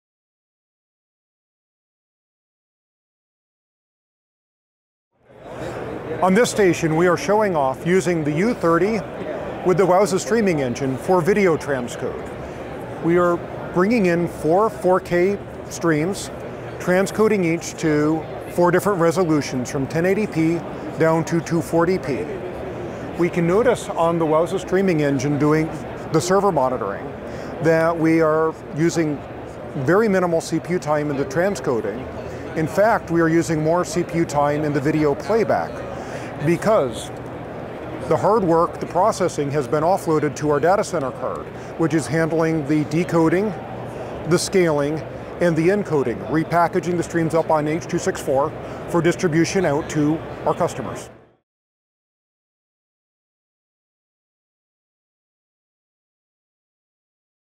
AMD Alveo U30 Media Accelerator Demo
Video Marketing Engineer, David Peascoe demonstrates the AMD Alveo U30 media accelerator. A data center product for real-time cloud streaming for interactive, immersive content.